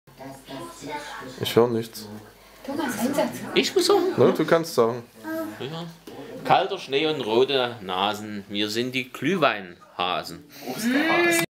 Nachwuchsabteilung verschmeckt Walthers Glühwein
Glühwein tut den Kinden gut, wenn die Eltern trinken gut... sehr lecker das Heißgetränk an kalten Tagen.